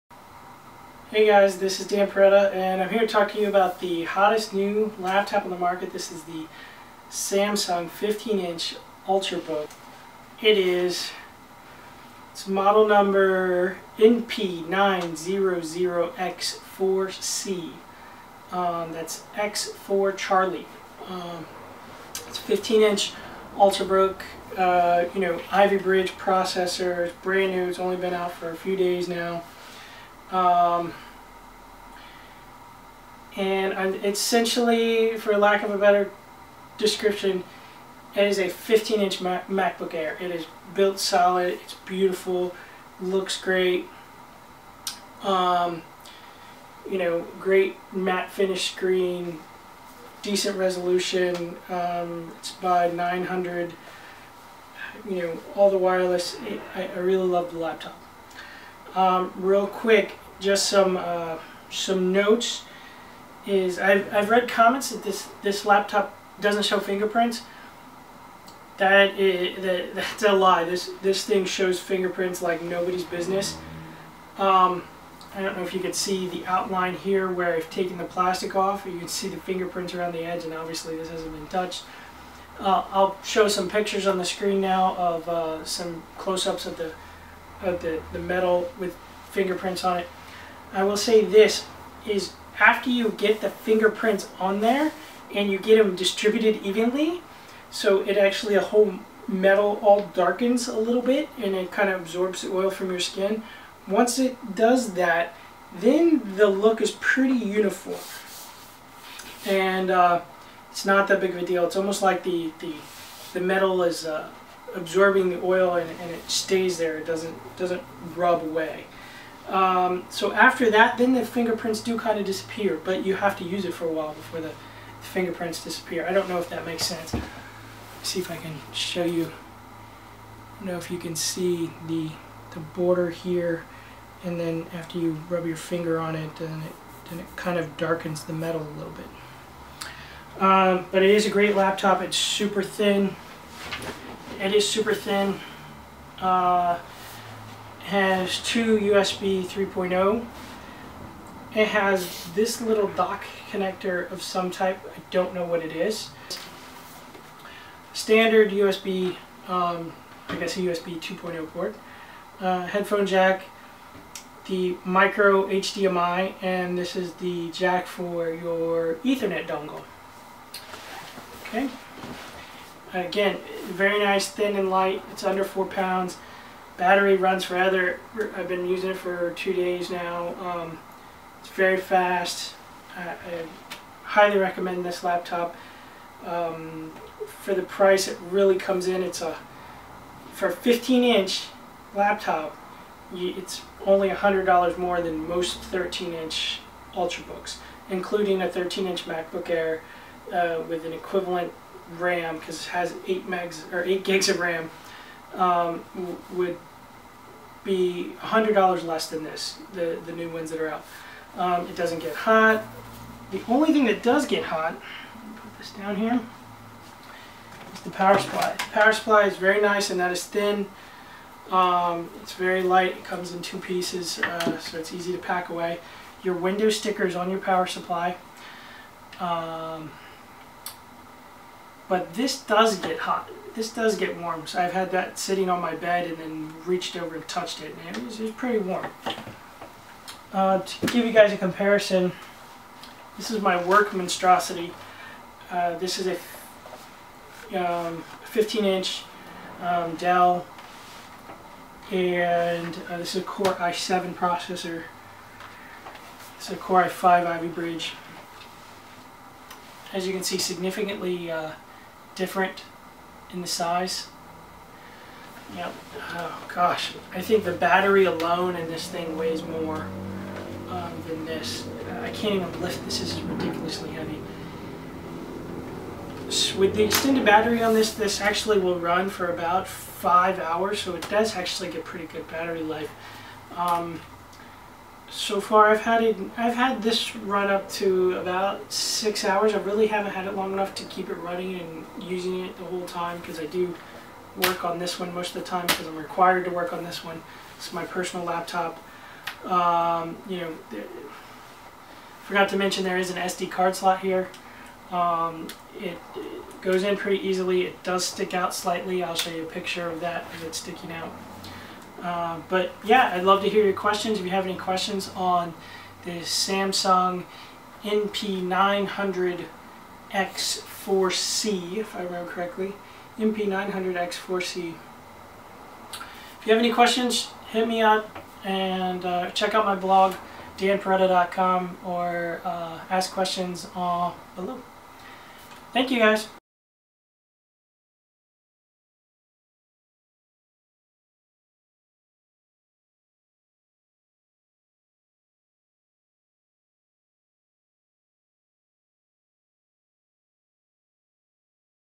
Samsung Series 9 15" Ivy Bridge ultrabook review - NP900x4c
a quick look at my new Samsung Series 9 15" ultrabook review -NP900x4c